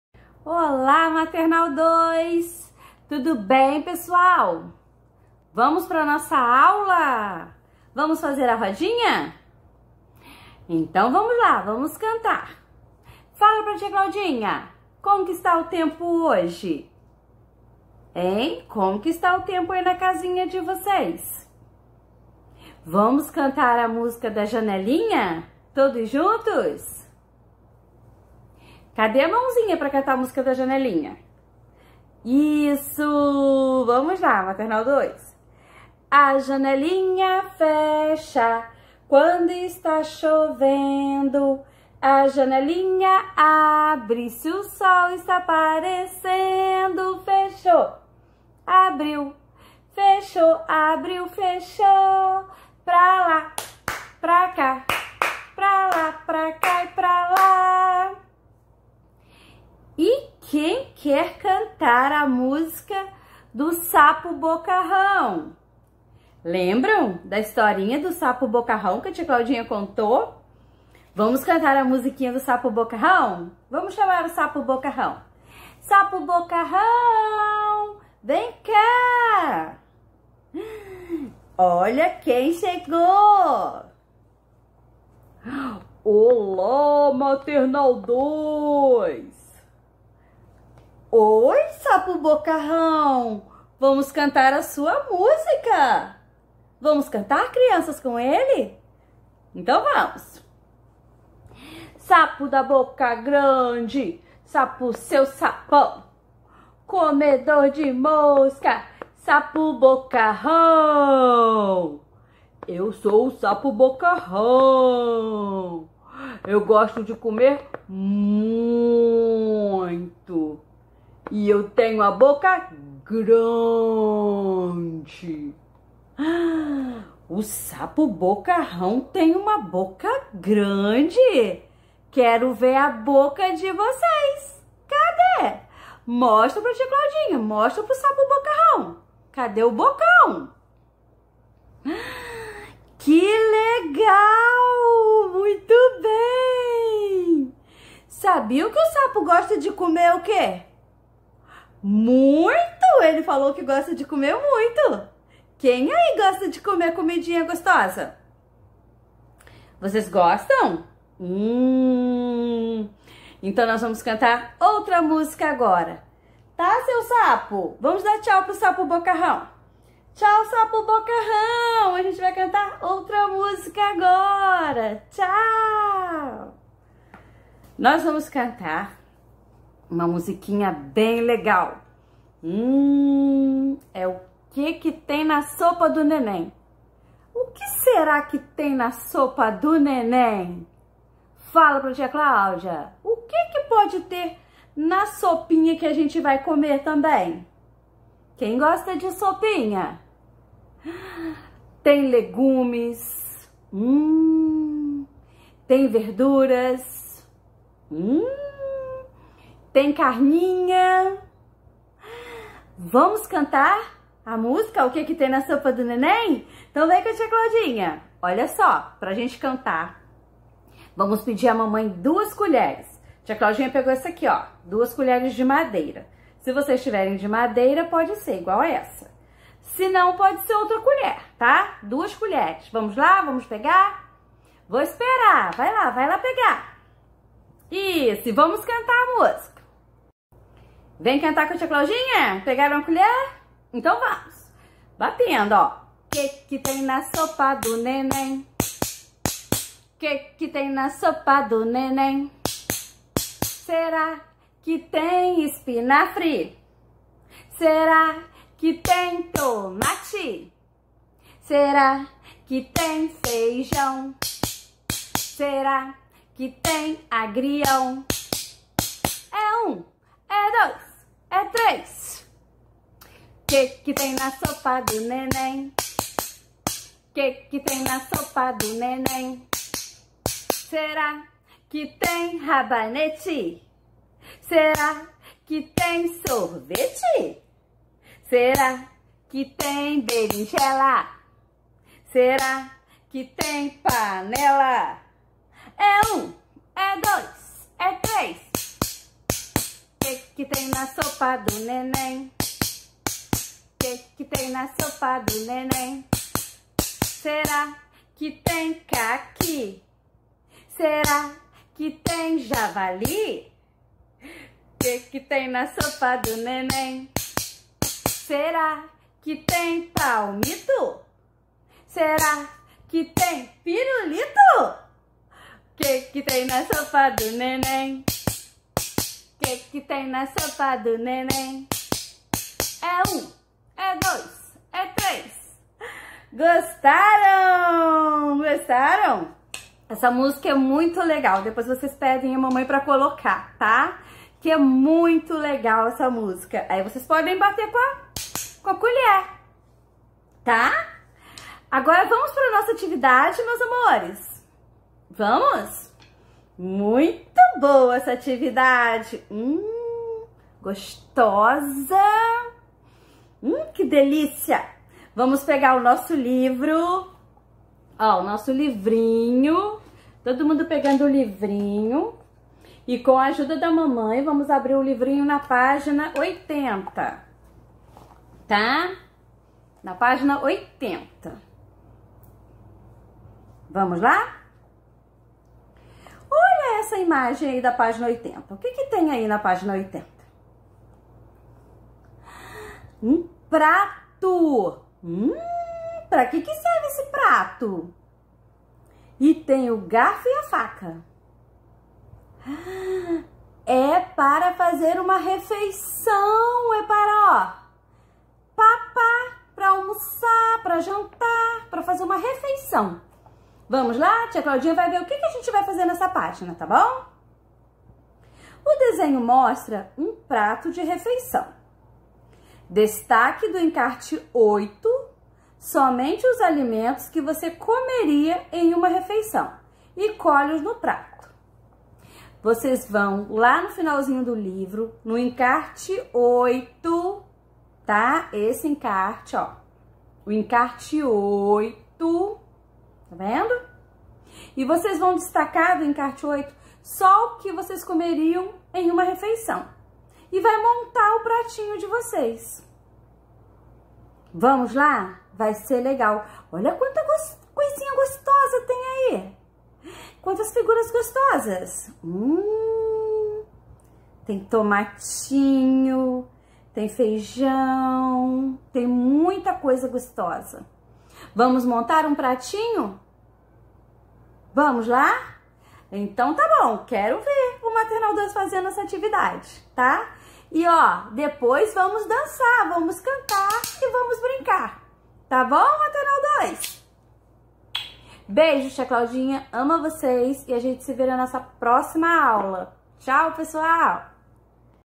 Maternal II - Aula 95 - Professora Cláudinha - Aula 02/12
Hoje nosso momento está bem legal,vamos cantar e nos divertir em nossa rodinha.
Que tal utilizar colheres para fazer som e cantar!? 😀
A atividade de hoje é na página 80 do nosso livro.
 Divirtam -se com a aulinha, crianças! ❤
 Beijinhos carinhosos, tia Claudinha.